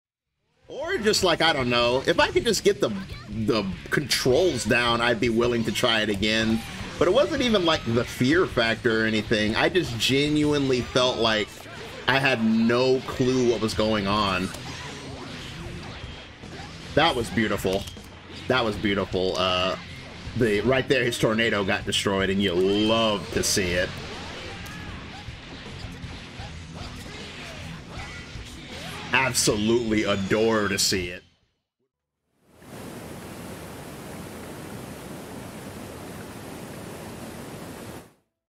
you absolutely LOVE to see it
For context, I am talking about the game "The forest" which I got destroyed on the previous stream lol!